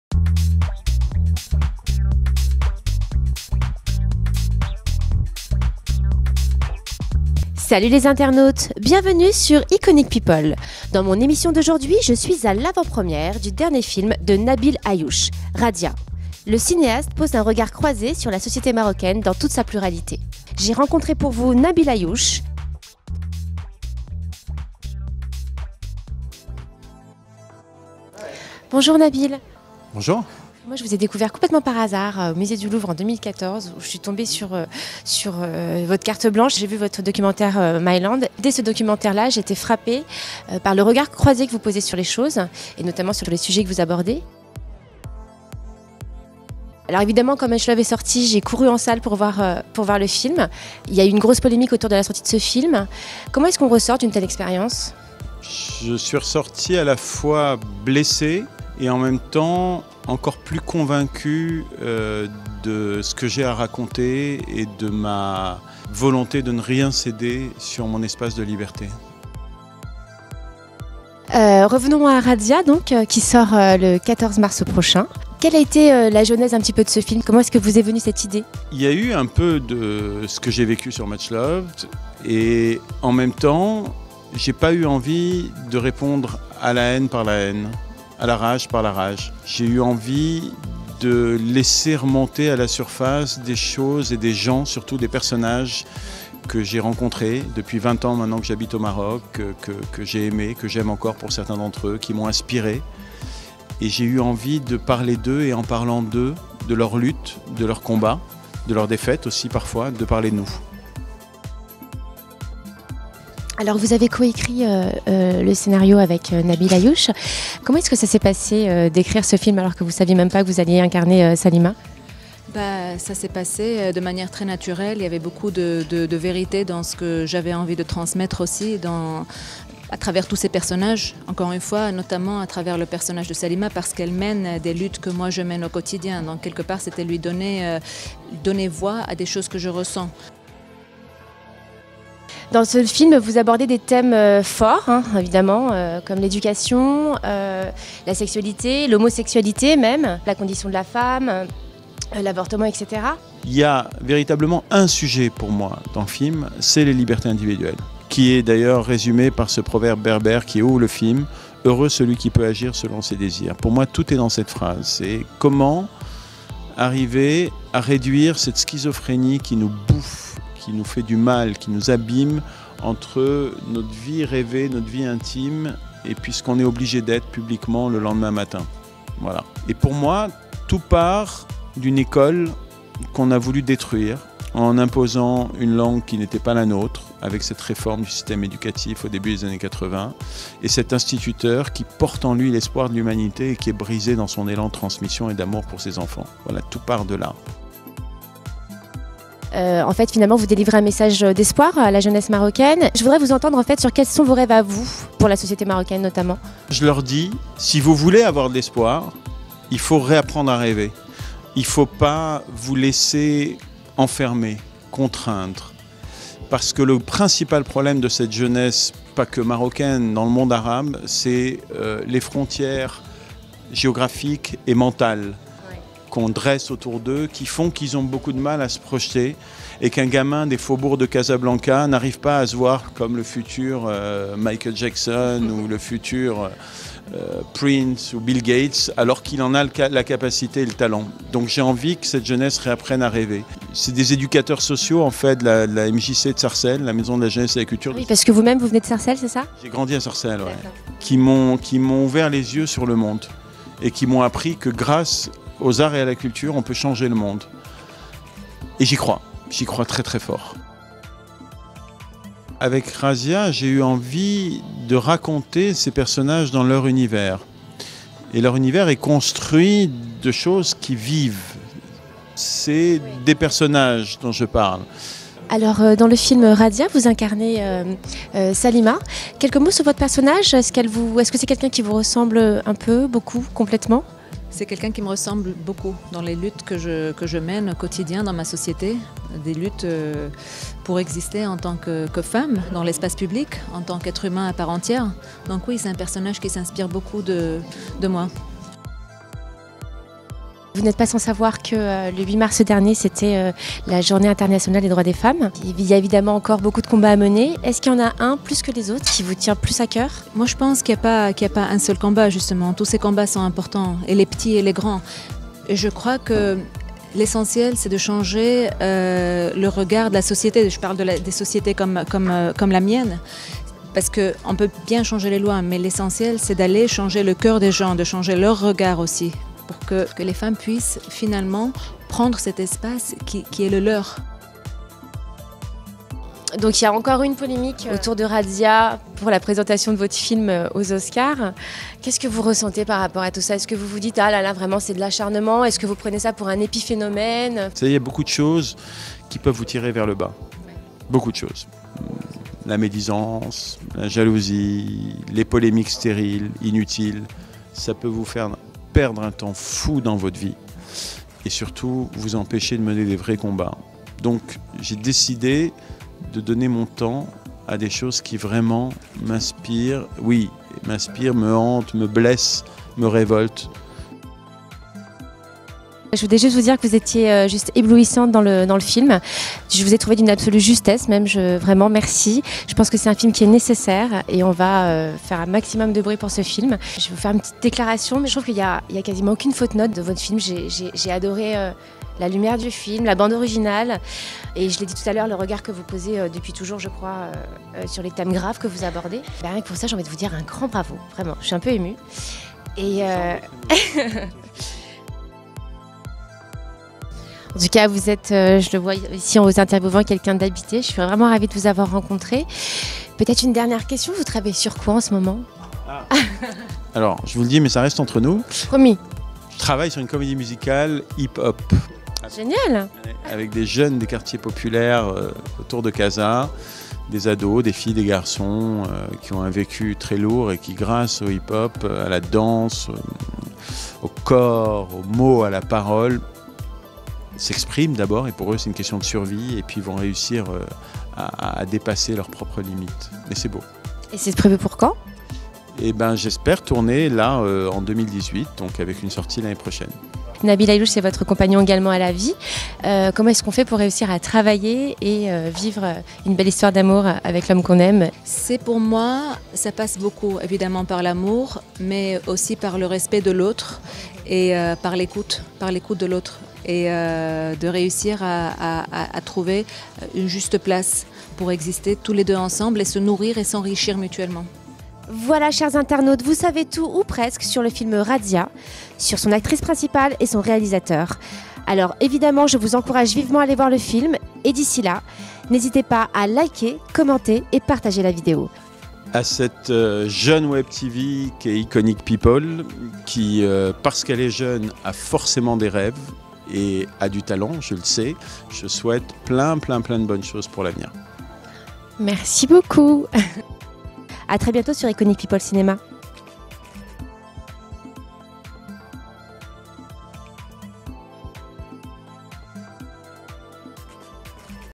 Interview - Maryam Touzani / Nabil Ayouch (Razzia)
Iconic People a rencontré Nabil Ayouch et Maryam Touzani à l'issue de leur nouveau film "Razzia".

A coeur ouvert, Nabil Ayouch s'est confié sur son film, ses personnages, leurs combats et partage avec nous ses convictions.

Un homme habité emprunt d'une grande générosité et doté d'une belle intelligence du coeur et de l'esprit !